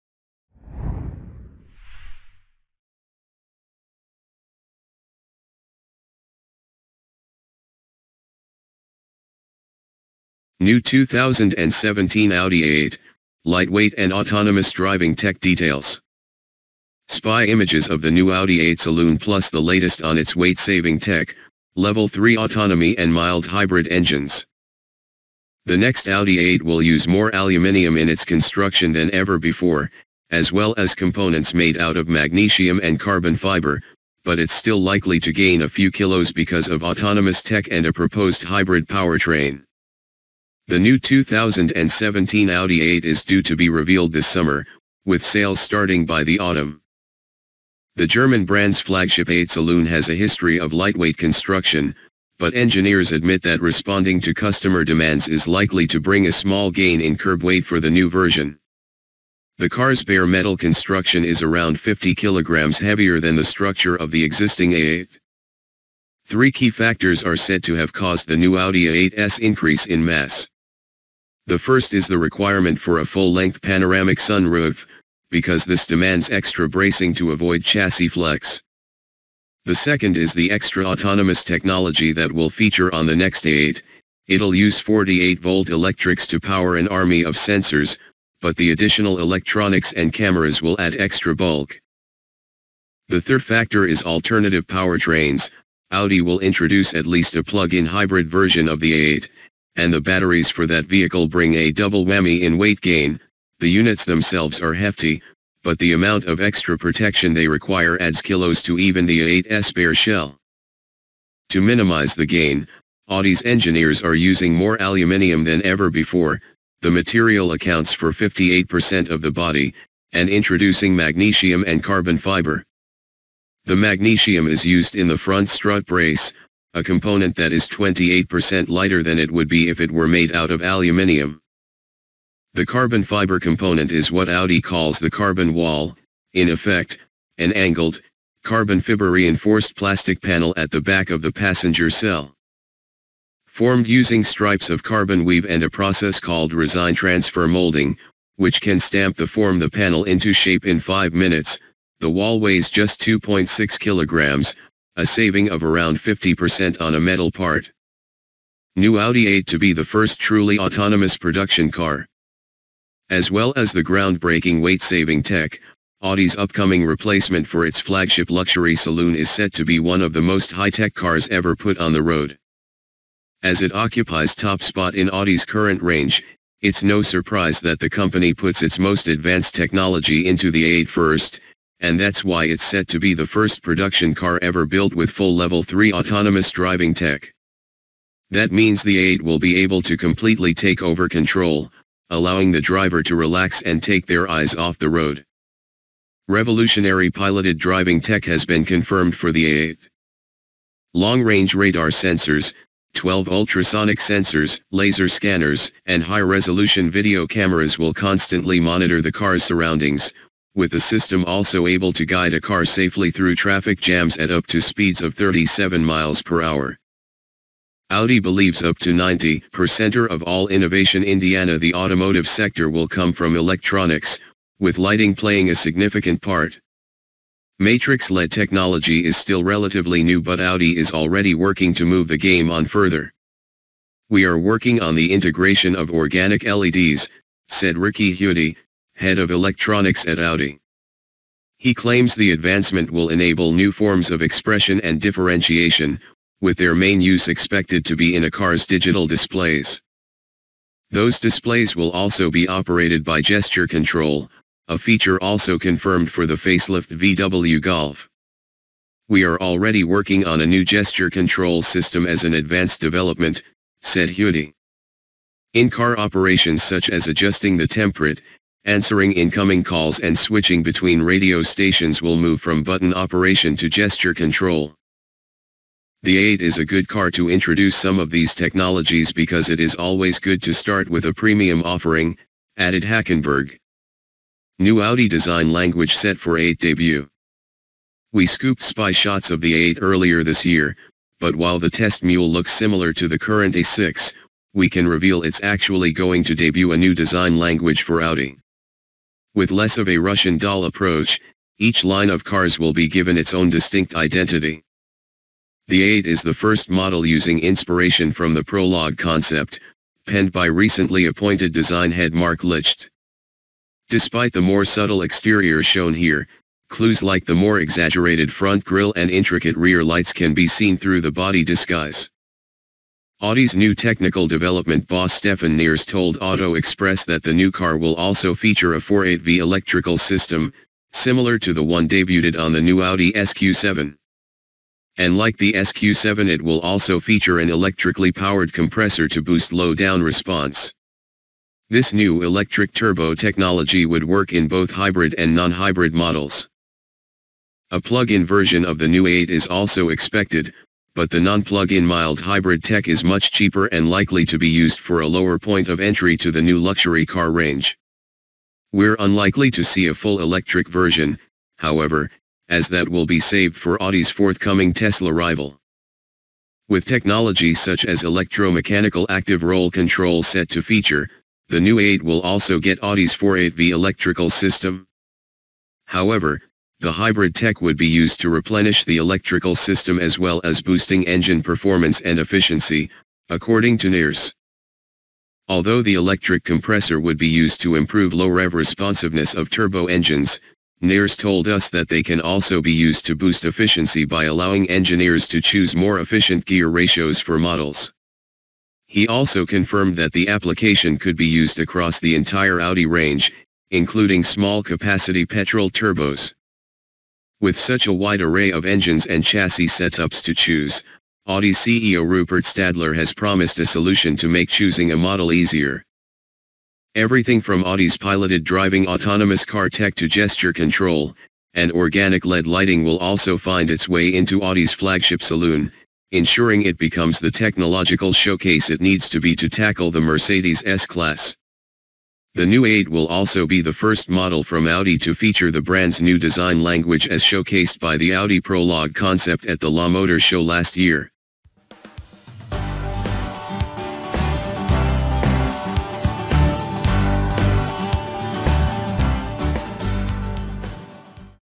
Car Review - New 2017 Audi A8: Light Weight And Autonomous Driving Tech Details - Read Newspaper Tv
Read newspaper:
Car review - New 2017 Audi A8: light weight and autonomous driving tech details
Source: autoexpress.co.uk